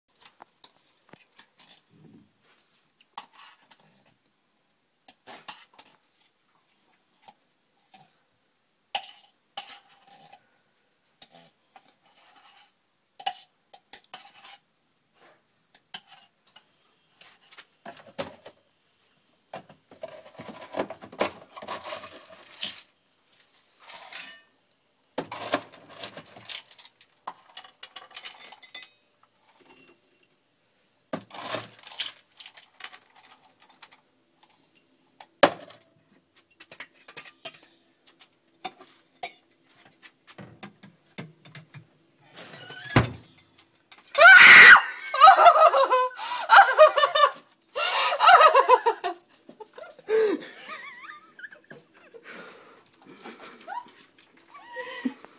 Friday 13th 2011
It's Friday 13th lily gets a scare!